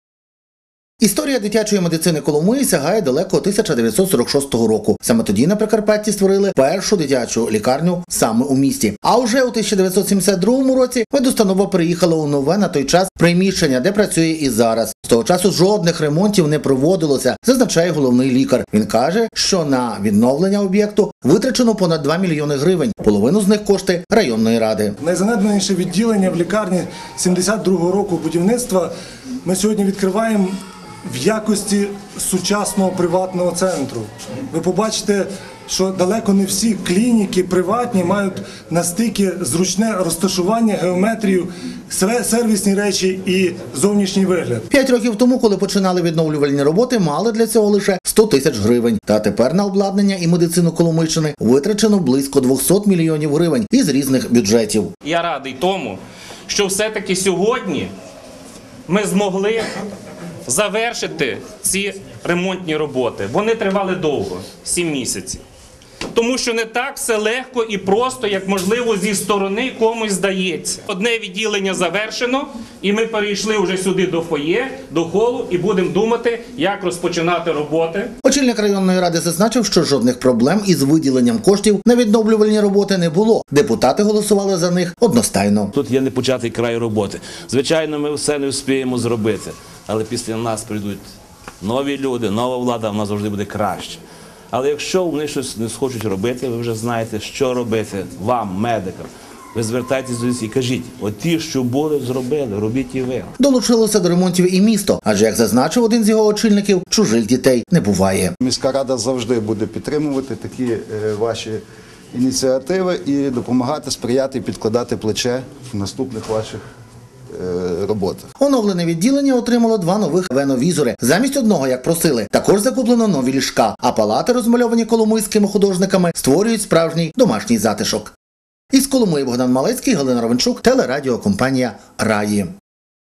У Коломиї оновили педіатричне відділення дитячої лікарні
Впоминатися за своє закликають медпрацівників Коломийської дитячої лікарні і місцеві чиновники, якщо раптом влада не допомагатиме їм. Про це говорили на відкритті відремонтованого відділення лікарні, яка понад 40 років не знала ремонтів.
Першим відновили педіатричне відділення. Тепер тут усе нове, а про обладнання та меблі подбали меценати.